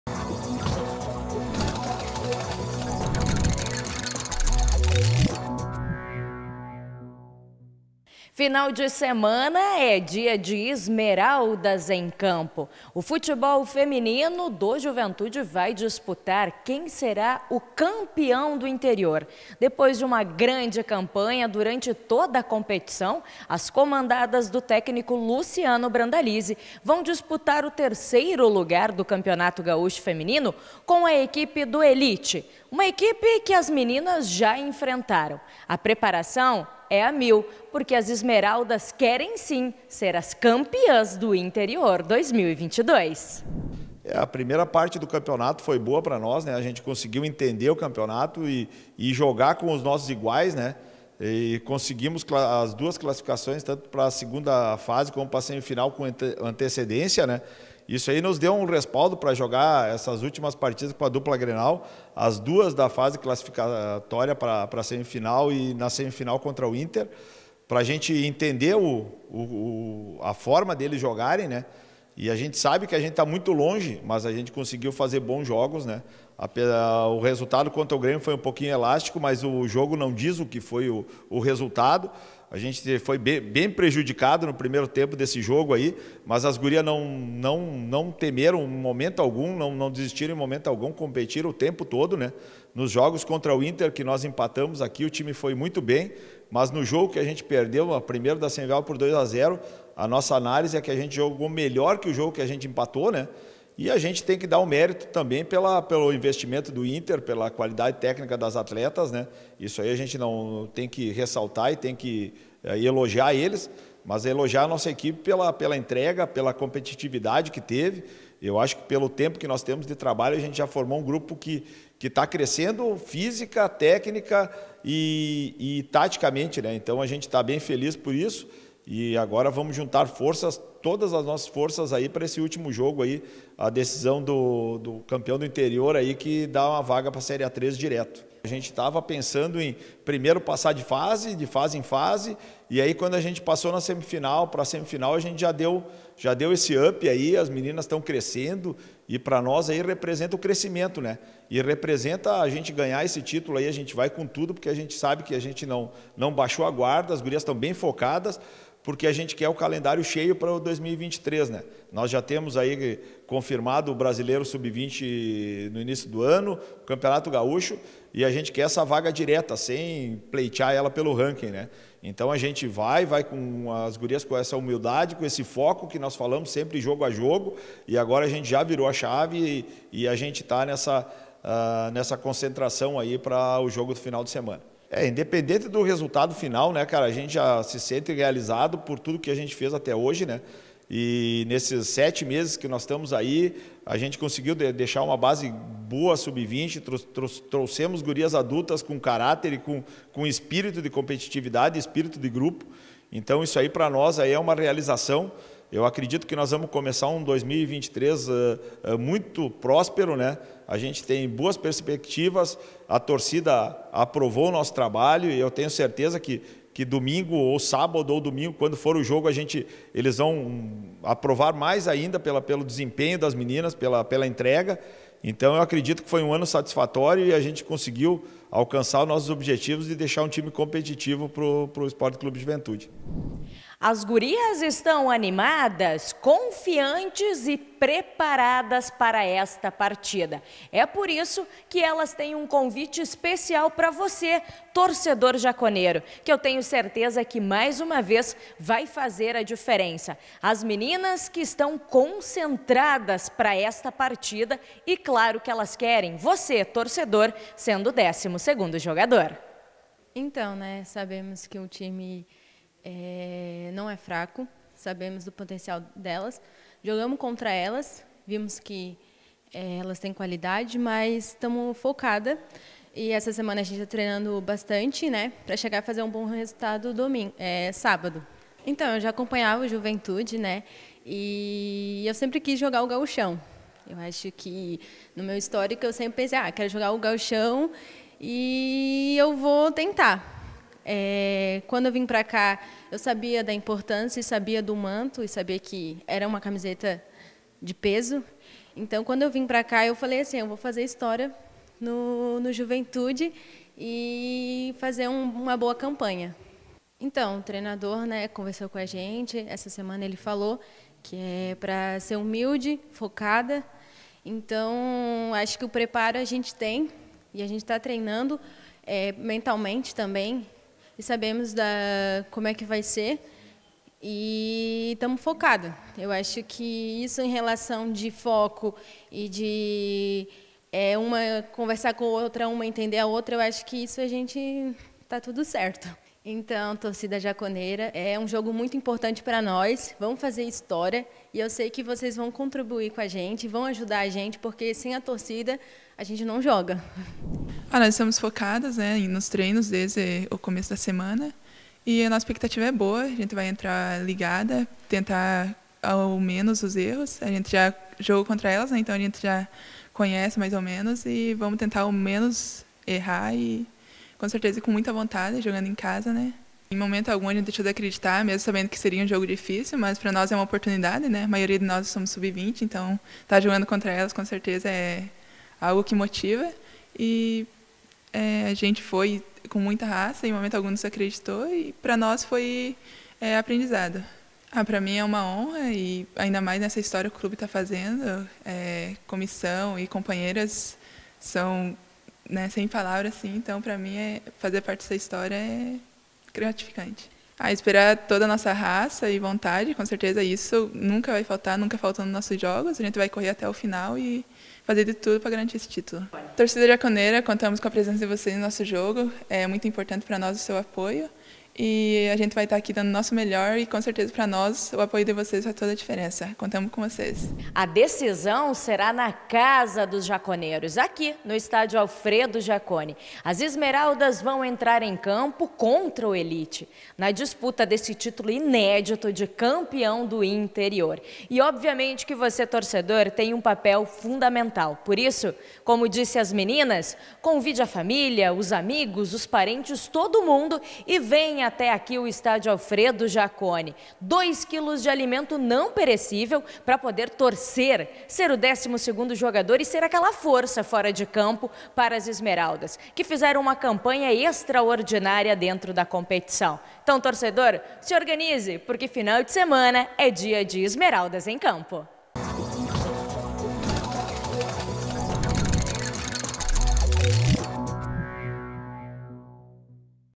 Esmeraldas decidem título do interior do Estadual
As gurias do Juventude disputam o título de Campeão do Interior do Campeonato Gaúcho no domingo (06.11), no Alfredo Jaconi. A TVPapoByXplay traz os detalhes dessa semana de preparação para esse confronto.